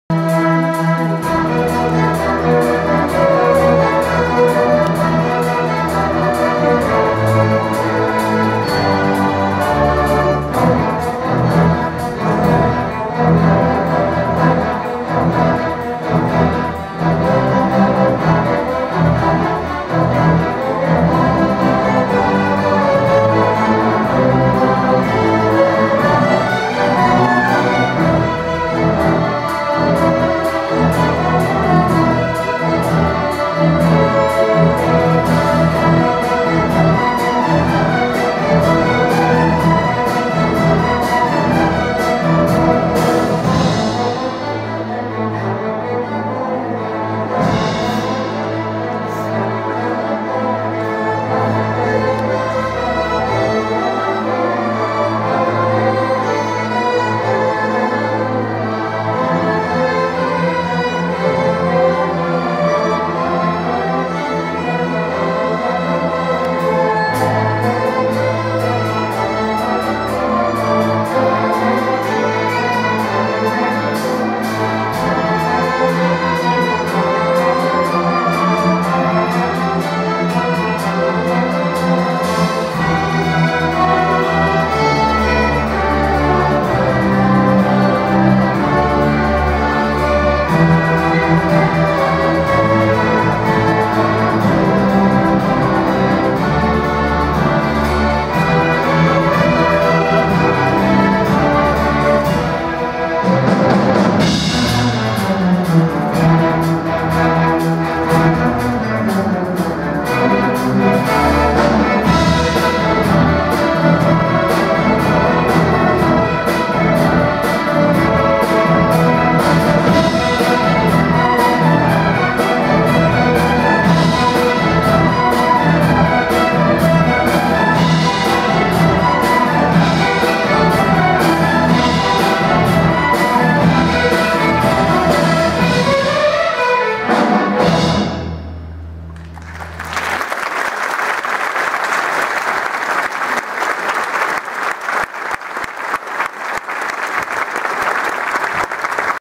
Wheeling Park High School 2022 Festival of Sound S1010005
The Wheeling Park High School Music Dept. combines together to perform their Christmas Concert known as "The Festival of Sound". These are few of the numbers performed during the 2 hour concert.